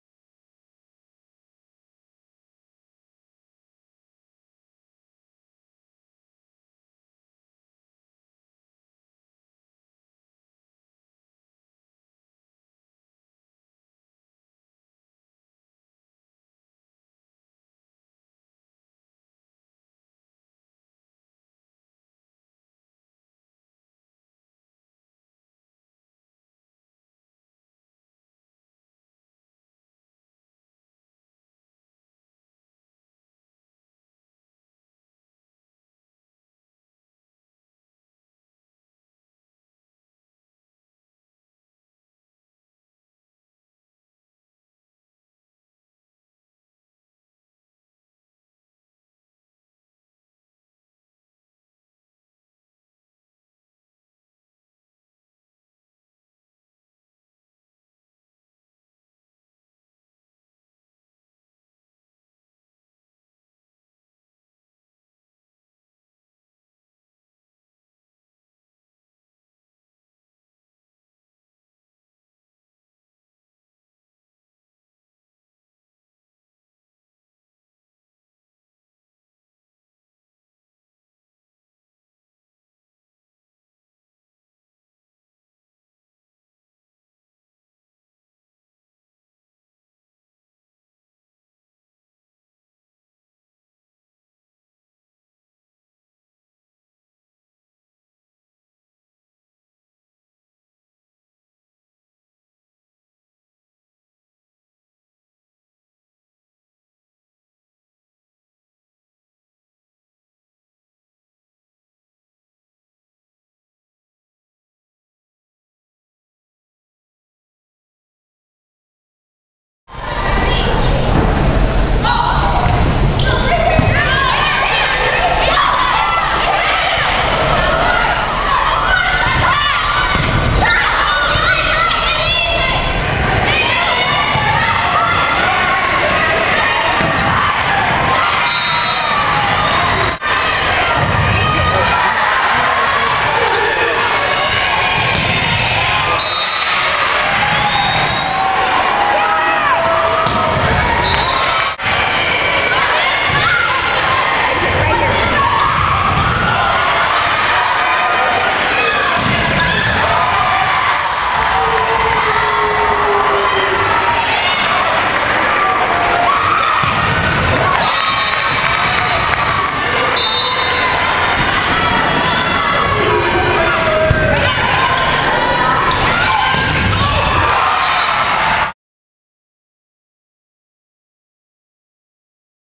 Bre Gibbons Skills and Highlight film
Bre Gibbons, senior outside hitter for Churchill High School and Webfoot 18 Gold.  Bre was the Midwestern player of the year and  first team all state.